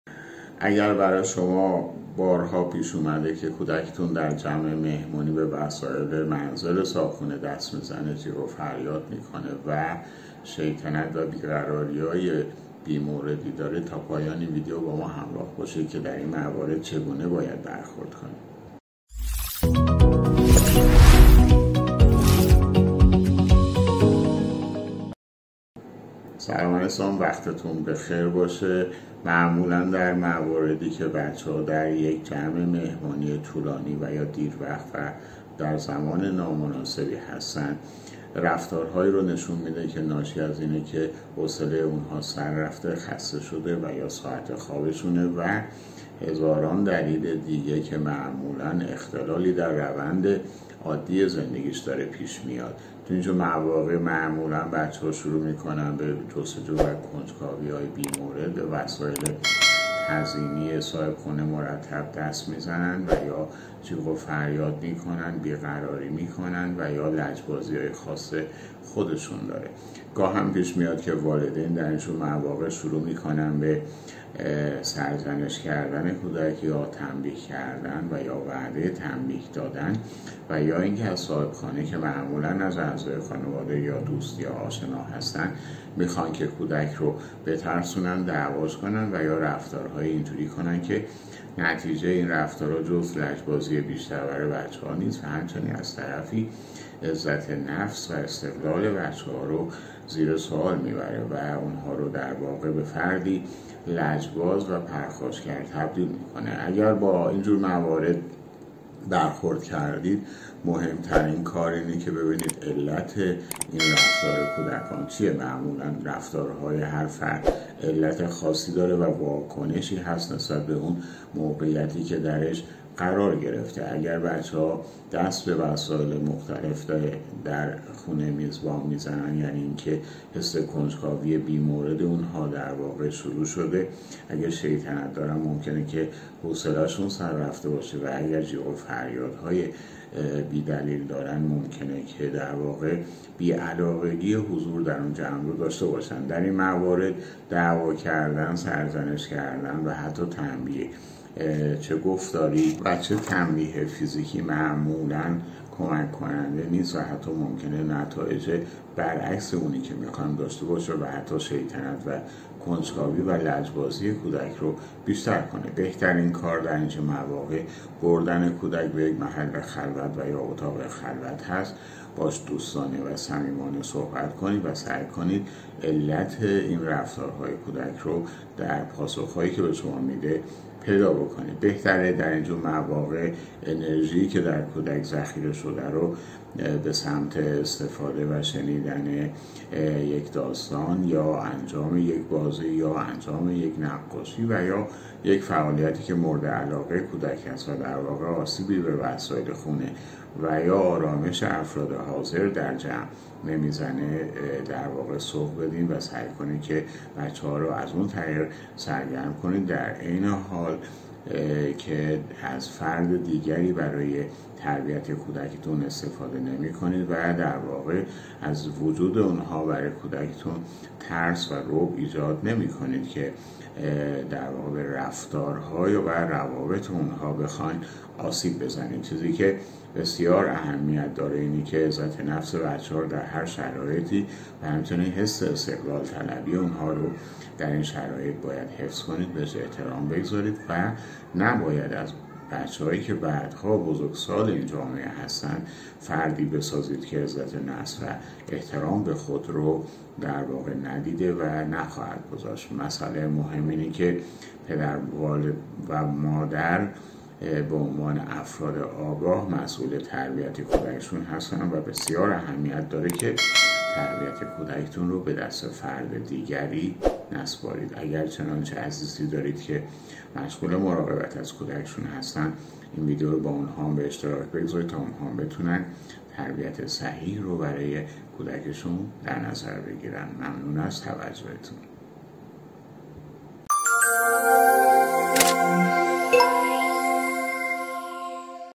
برخورد با شیطنت کودکان در مهمانی
سلام وقت بخیر همراهان گرامی🌺🌺🌺🌺🌺🌺🌺🌺🌺🌺🌺🌺🌺

 کودکان از زمانی که به دنیا می آیند الگوهای رفتاری خود را از والدین و مراقبین خود دریافت می کنند😍😍😍😍😍😍😍

آنها در برخورد با مشکلات دنیای بیرون ،به واکنش ها و عکس العمل های بزرگترها توجه و اعتماد کنند🥰🥰🥰🥰

 بنابراین, همه رفتارهای بزرگترها ،برای آن‌ها جنبه آموزشی دارد،😍😍😍😍😍😍

 اگر چنانچه اشکال رفتاری در آنها مشاهده می کنید به دلیل اشکال در رفتارهای والدین است🙄🙄🙄🙄🙄🙄

 یکی از بدترین نوع برخورد با اشتباهات رفتاری کودکان برخورد عصبانی با آنهاست😔😔😔😔😔😔

 در این گونه موارد ،ناخودآگاه به کودک یاد می دهید که در برخورد با ناملایمات و اشتباهات رفتاری دیگران ،با خشم و غضب واکنش ،نشان دهند و حتی می تواند دیگران را تنبیه کنند😮😮😮😮😮😮

کودکان در زمان عصبانیت شما،متوجه نکات آموزشی و اعتراض شما نمی شوند،تنها صدای بلند شما را می شنوند و درک درستی از آن نخواهند داشت 😔😔😔😔😔😔😔😔😔

  اگر چنانچه اشکال رفتاری در آنها مشاهده می کنید به دلیل اشکال در رفتارهای والدین است،والدین عصبانی ،کودکان پرخاشگر یا خجالتی پرورش می دهند😳😳😳😳

مراقبت_نوزادان_ کودکان
راههای ارتباطی با دکتر محمد منصوری متخصص نوزادان و کودکان